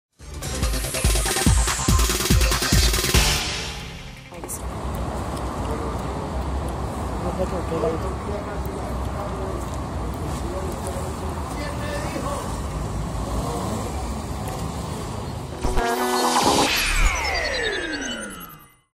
Finaliza levantamiento del cuerpo del hijo de Navarro Wolf
El Laboratorio Movil de Criminalística de la Policía Nacional, finalizó la tarde de este miércoles el levantamiento del cuerpo de Gabriel Navarro, hijo del senador Antonio Navarro Wolf, quien falleció pasada la 1:00 pm este 28 de enero.